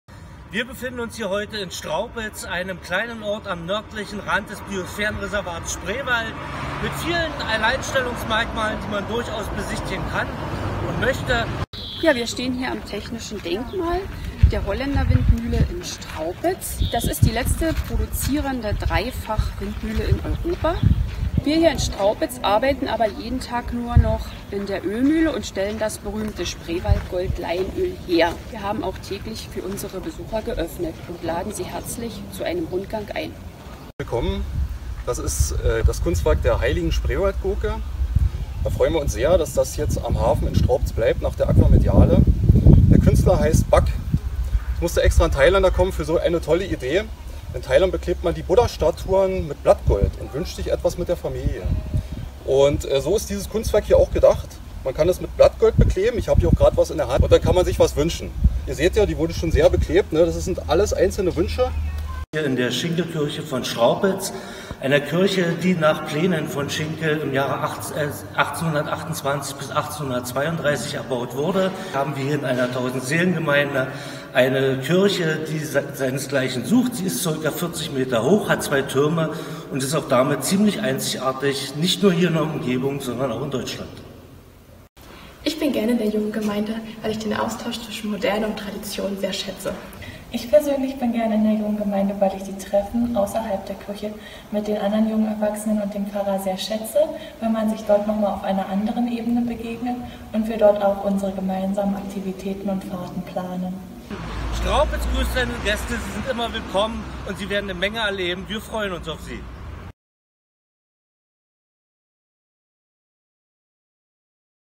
Virtuelle LR Tour Straupitz
Reisen in Corona-Zeiten - wer nicht ins Ausland will, entdeckt heimische Ziele. Während man in Brandenburg und im Spreewald vielerorts längere Strecken in Kauf nehmen muss, um von einer Attraktion zur nächsten zu finden, macht Straupitz im Oberspreewald mit seiner „Kulturmeile“ eine Ausnahme.